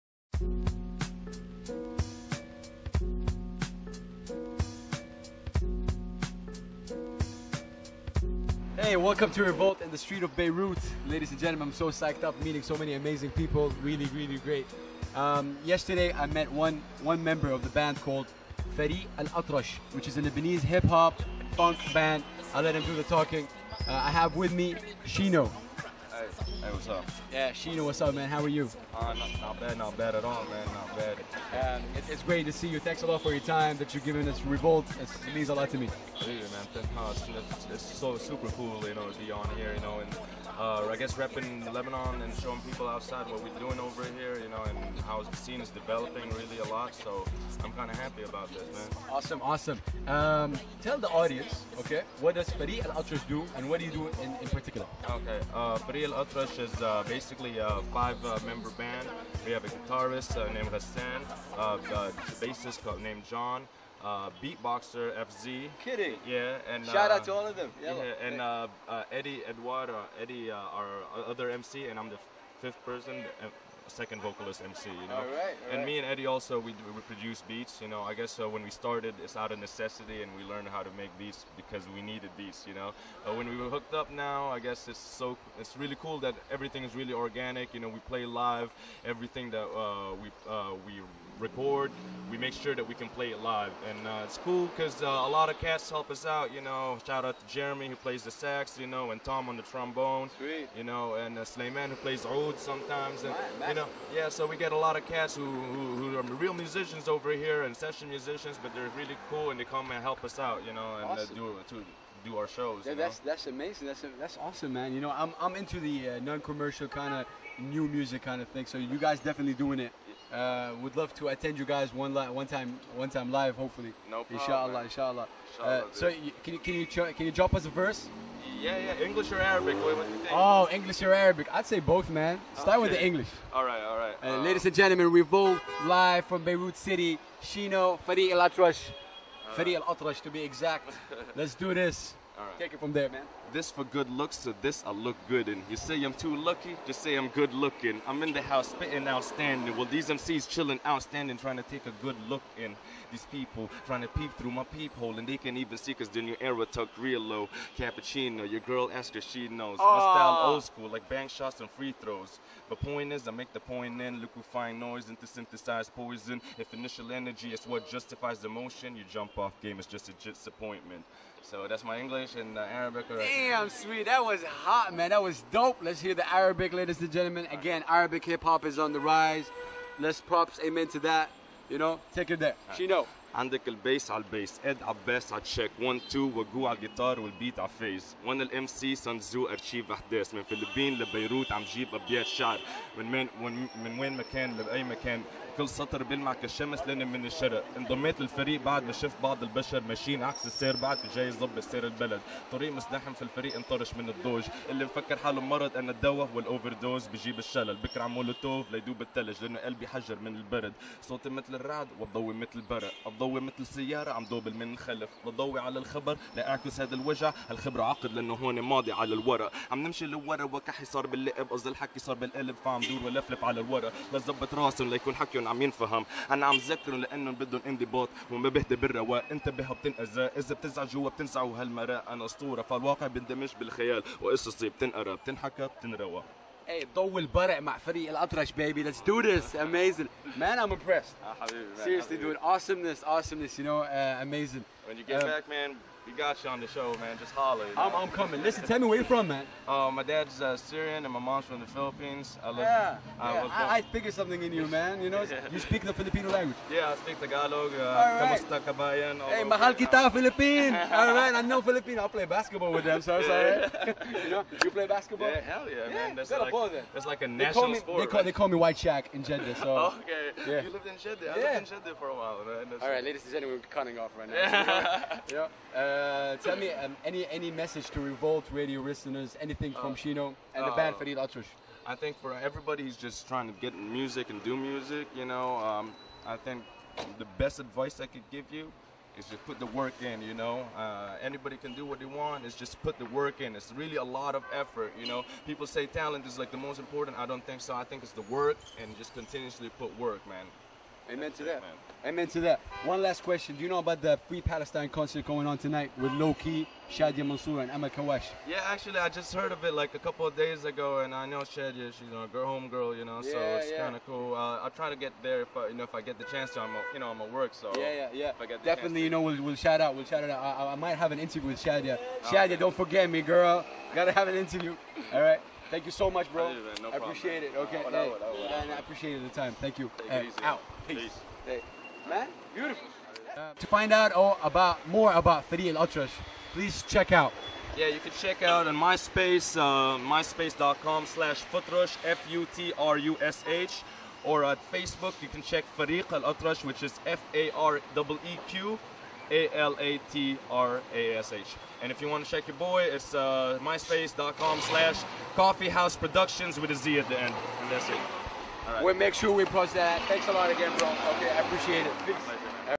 REVOLT RADIO INTERVIEWS CHYNO FROM FAREEQ AL ATRASH
During my LIVE coverage for the "Free Palestine" Concert that took place in Beirut. (AUB)
I managed to land an interview with Chyno, a member of the a Lebanese HIP HOP/Funk Band "FAREEQ AL ATRASH"

Check out the interview & the freestyle Chyno performed in the middle of Hamra Street!